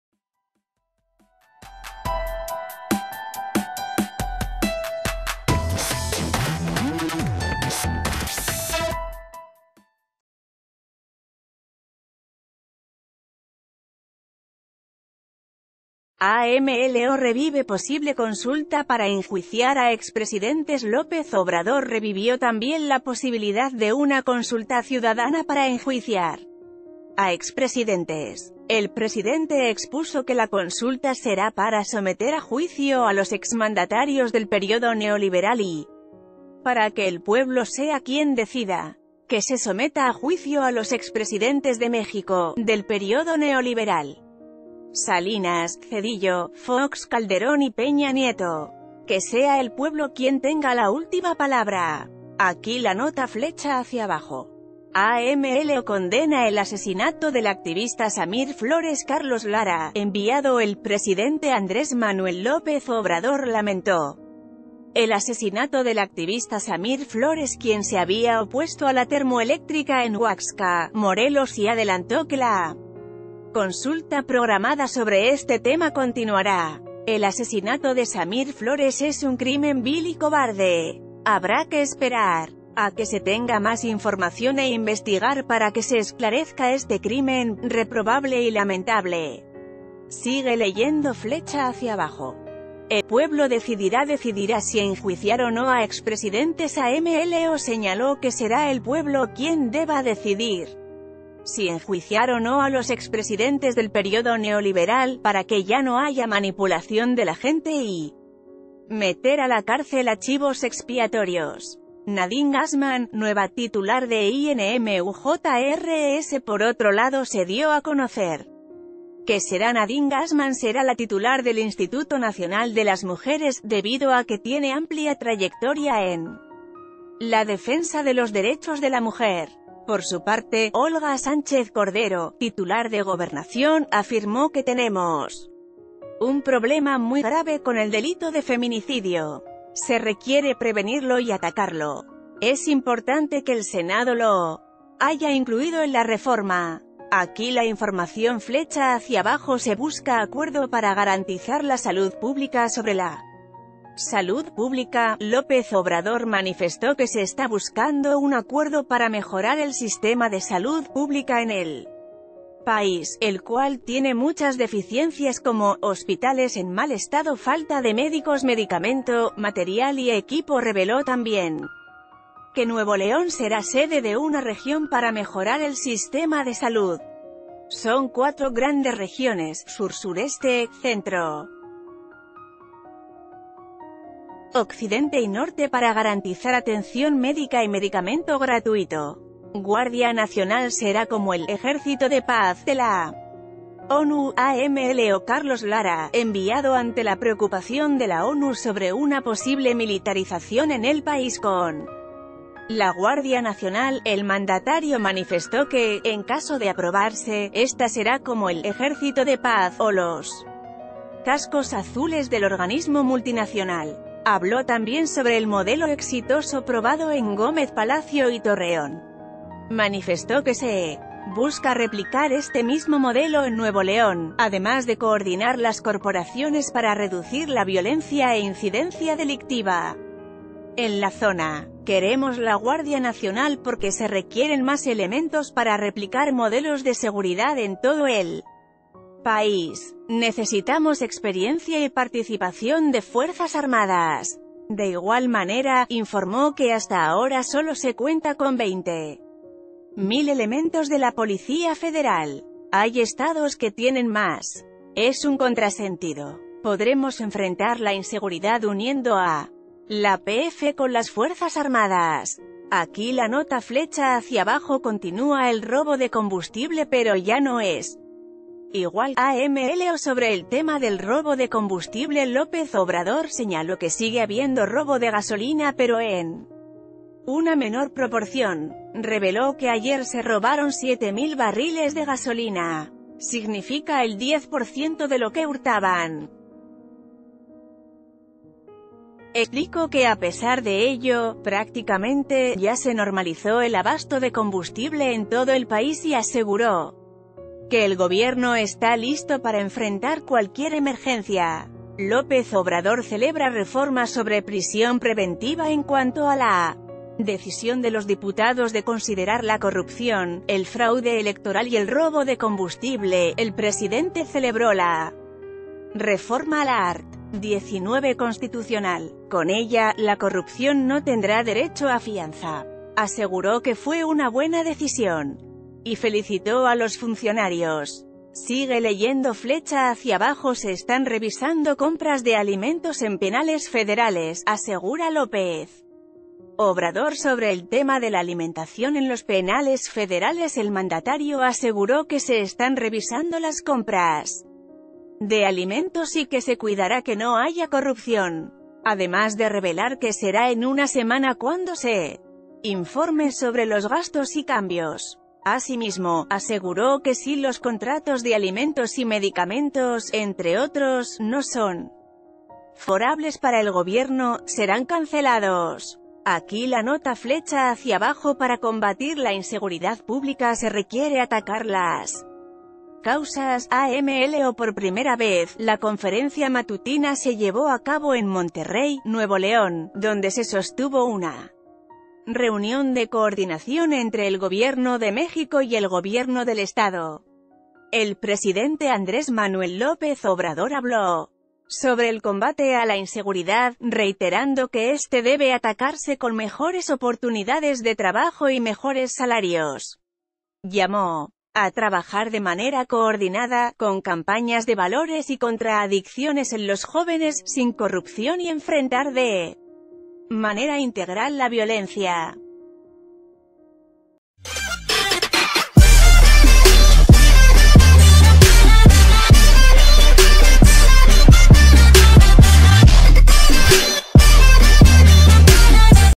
Sigue la agenda de AMLO este 20 de febrero
AMLO revive posible consulta para enjuiciar a expresidentes López Obrador revivió también la posibilidad de una consulta ciudadana para enjuiciar a expresidentes. El presidente expuso que la consulta será para someter a juicio a los exmandatarios del periodo neoliberal y para que el pueblo sea quien...